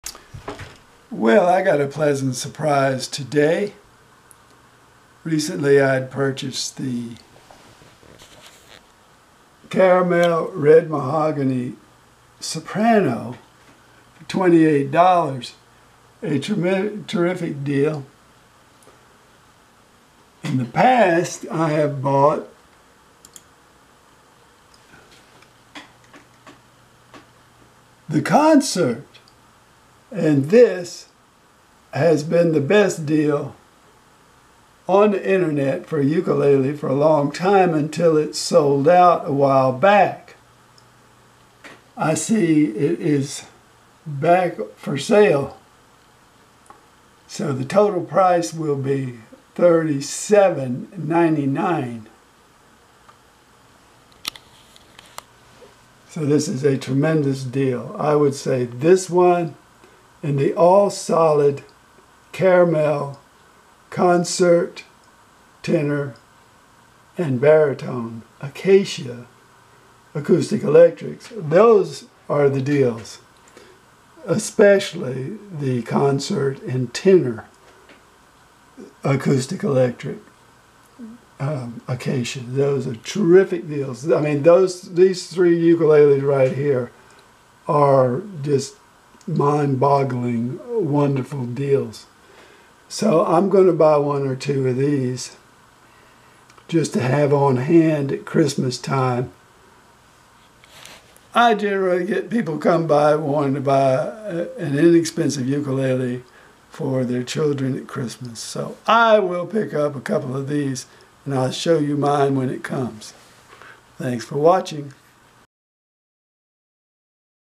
Rich Buys More Caramel Ukuleles! It's a Good Deal I Couldn't Pass!
Looks like Caramel red mahogany concert and Caramel all solid glossy acacia baritone are back in stock!  This concert is a top Amazon concert ukulele bargain.  I like the style, color, sound and playability of this ukulele.  I love the super smooth fretboard.  Price is crazy low!!
Gig bag is not so good but ukulele is worth 3 X what you pay.